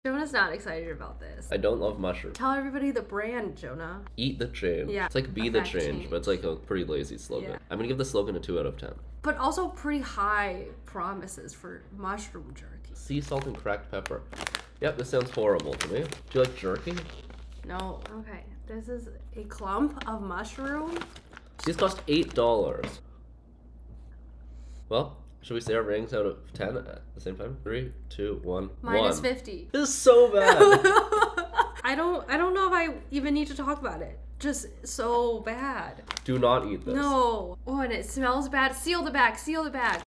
Two Haters Try Disgusting Mushroom Jerky From Most Expensive Grocery Store In America
Erewhon is expensive, but are the snacks any good? Not this one. We tested this snack with low expectations but even those were not met. We won't be eating Mushroom Jerky any time soon.